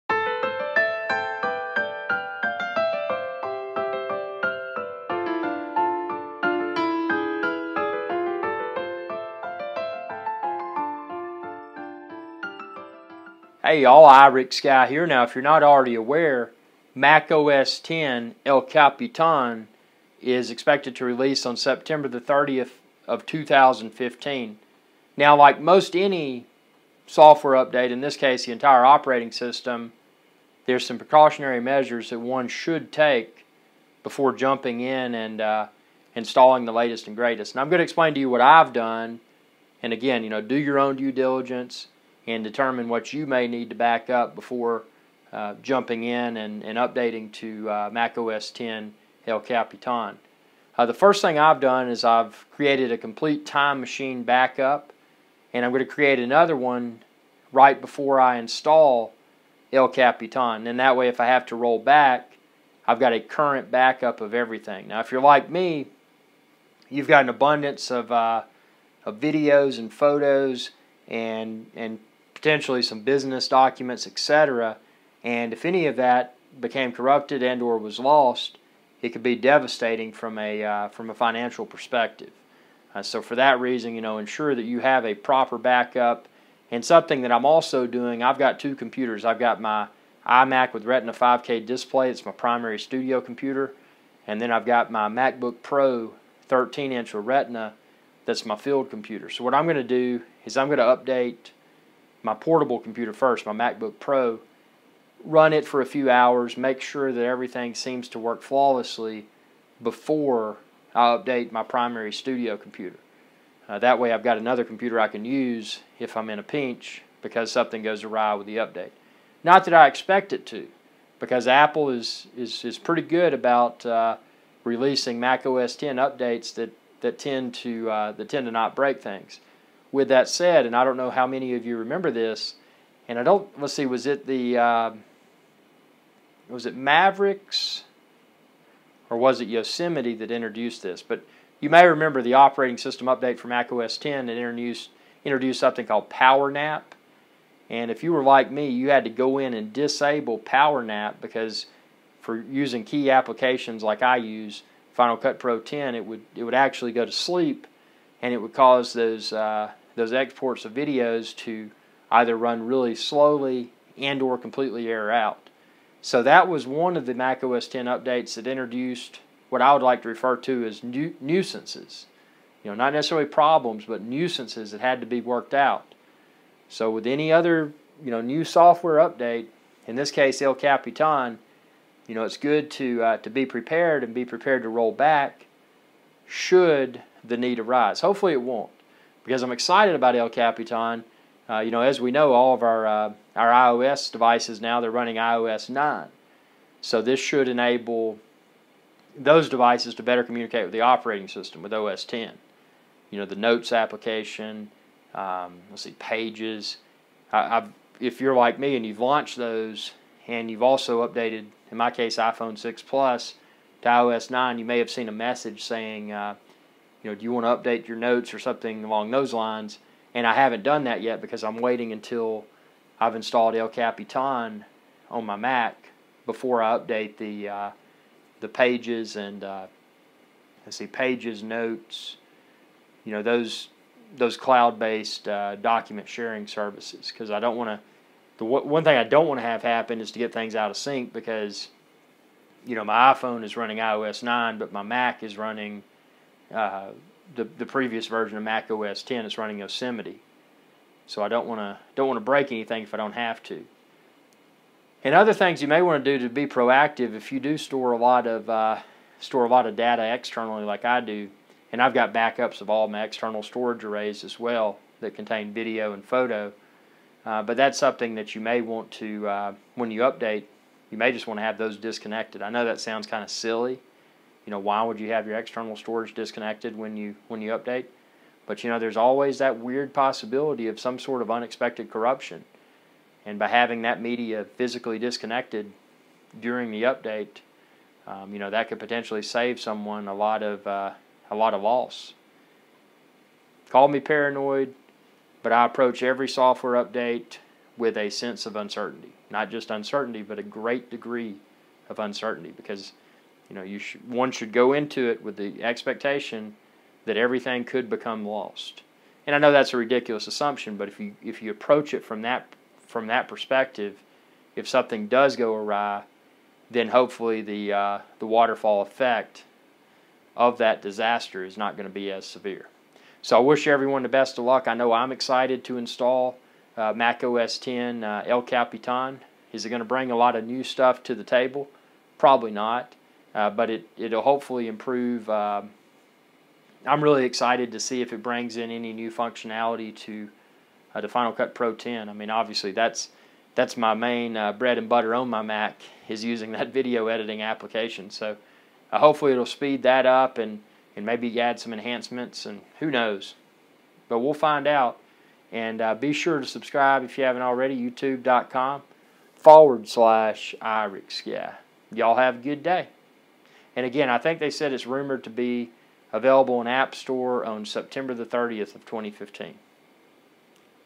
Mac OS X El Capitan Upgrade Preparations
Mac OS X El Capitan releases September 30, 2015.  These are some precautionary steps that I'm taking before upgrading my Macs to El Capitan.  Even though I don't anticipate any issues, it's always best to be proactive and expect the worst any time that a software update is installed.